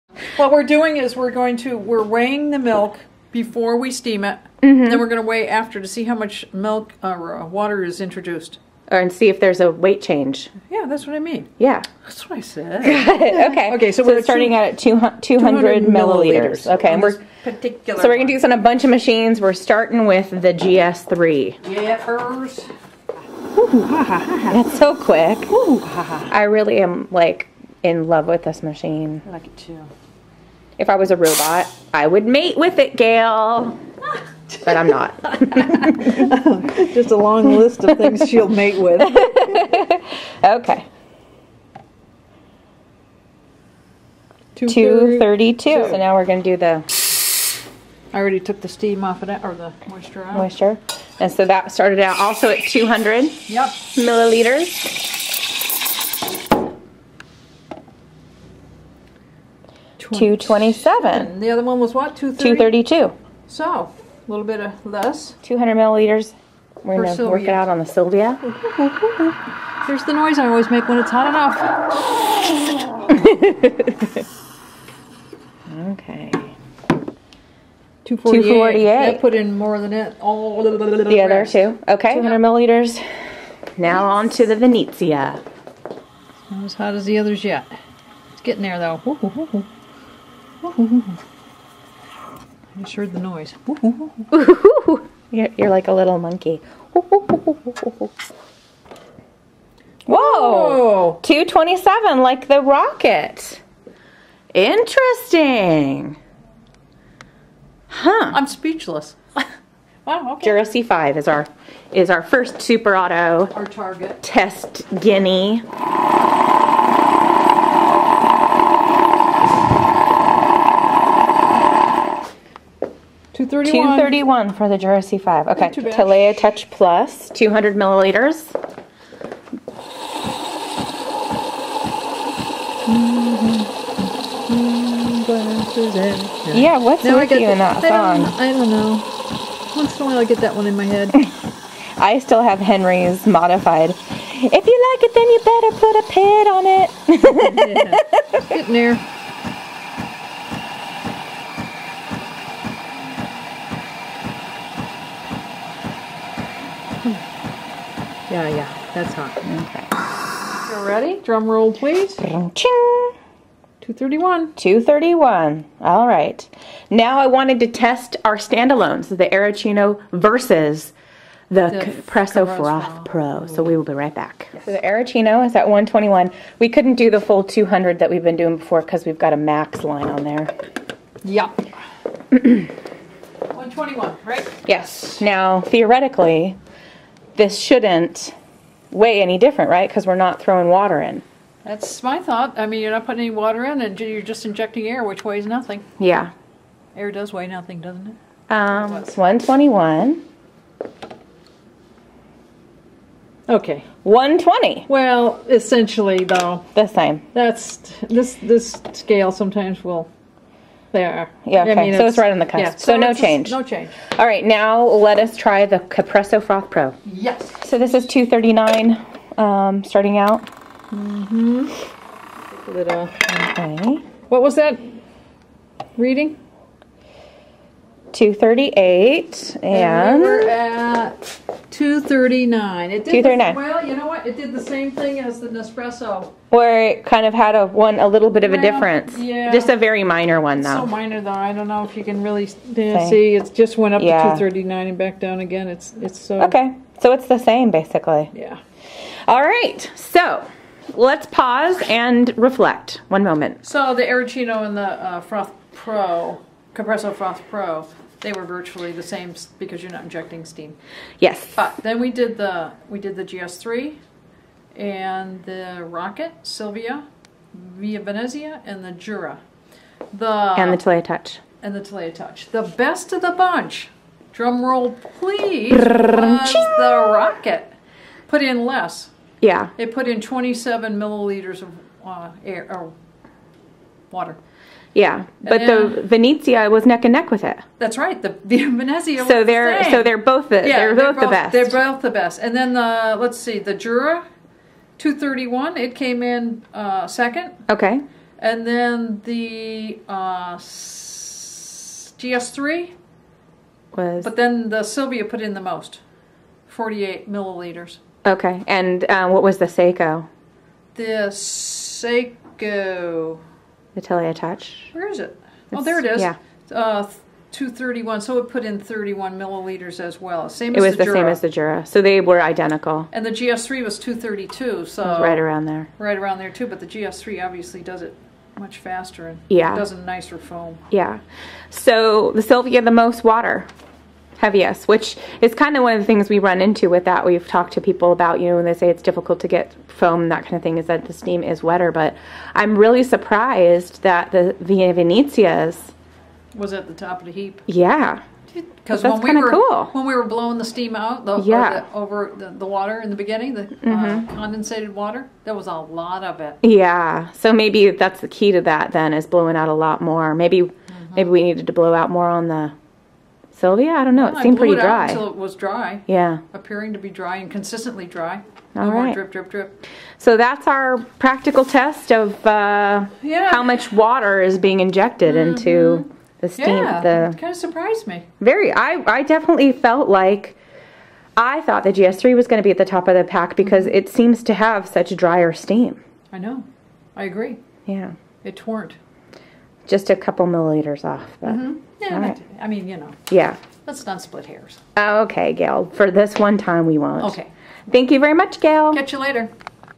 Testing: Milk Weight Before and After Frothing
We often are asked this question so decided to run it through a bit of a test. We weighed out the milk in the pitcher before steaming and then after steaming, seeing what, if any, difference there might be. Up on deck were the La Marzocco GS3, Rocket Giotto Evoluzione, Rancilio Silvia, Saeco Via Venezia, Jura C5, Saeco Talea Touch Plus and two stand alone frothers by Nespresso and Capresso.